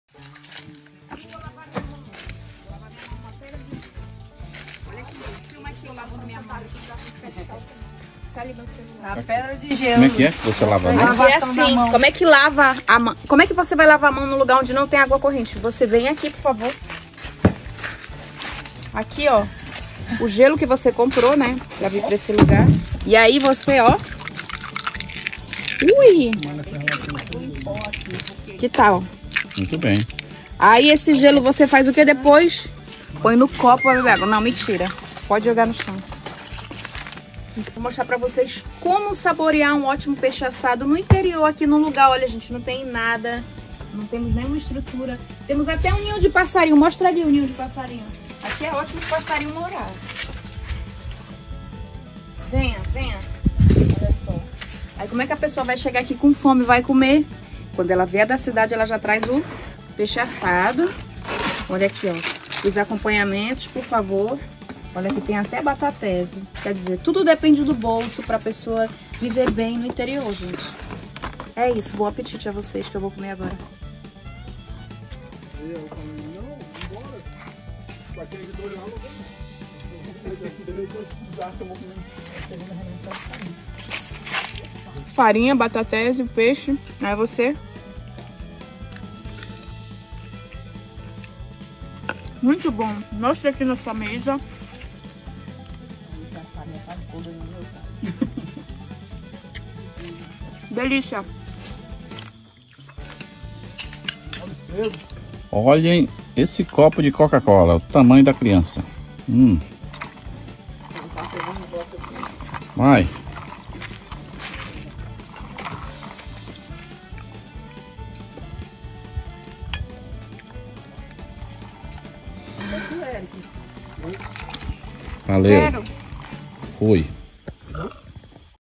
“Sobrevivendo” um dia na selva sem água corrente, copo e comendo um peixe assado
Fomos pro sítio, estava abandonado, nao tinha louça limpa, então improvisamos